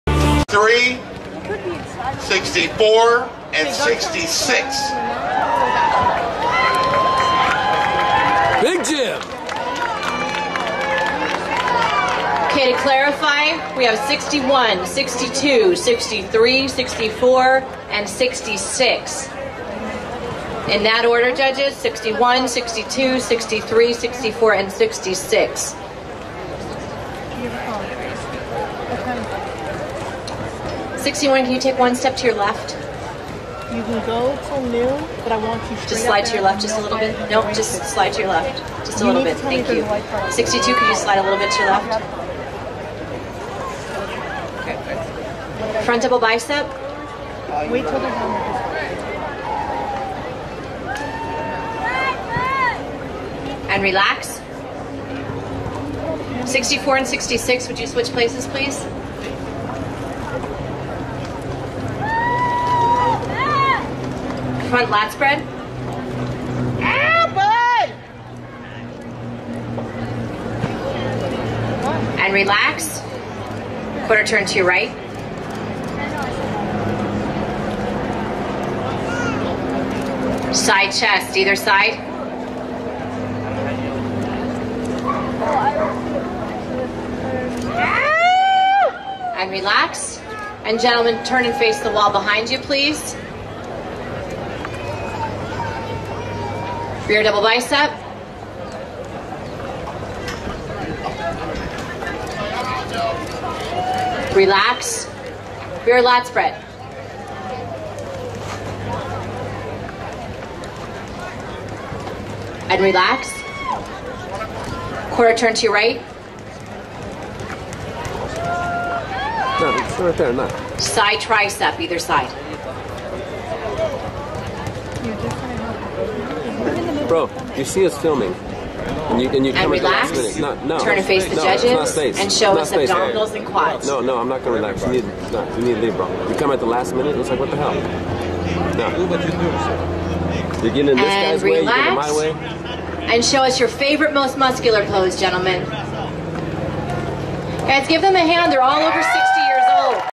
Cal Strong, age 80 in light blue shorts, competes on Muscle Beach
A video of the legendary Cal Strong